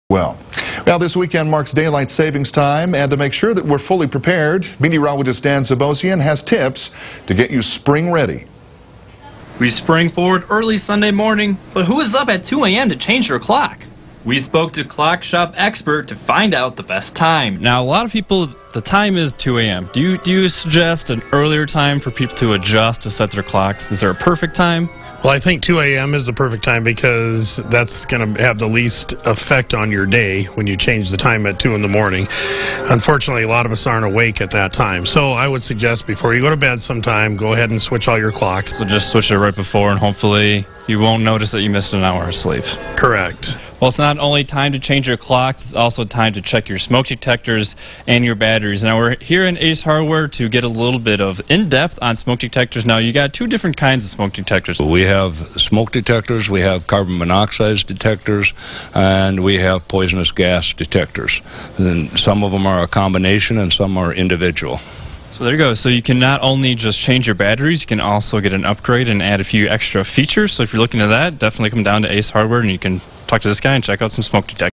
It's Time Again For Daylight Saving
This weekend marks daylight saving and Meteorologist Dan Zbozien says its time to get ready and spring forward!